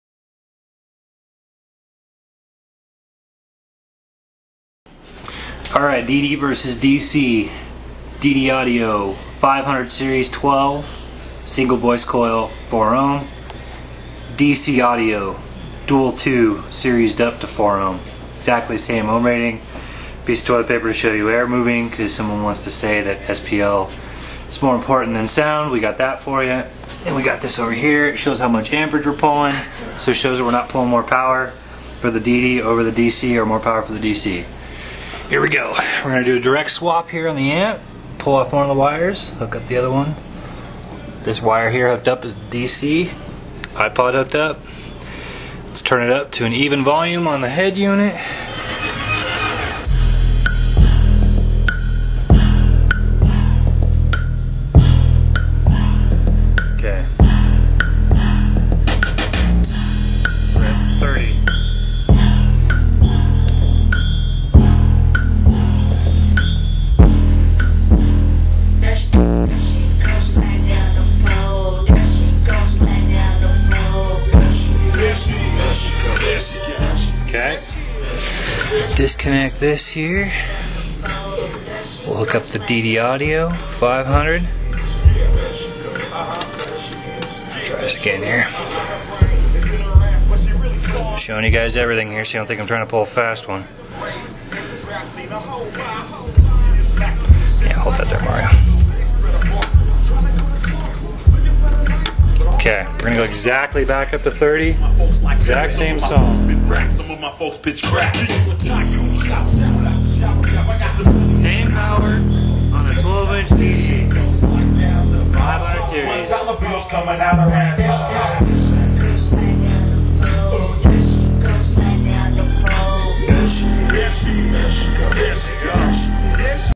DD audio 500 vs DC lvl 2
this is about 200-300 watts, same volume gain for each sub, same power going out to each sub. different outputs. controlled test results shown here. ready to test the big dc stuff now!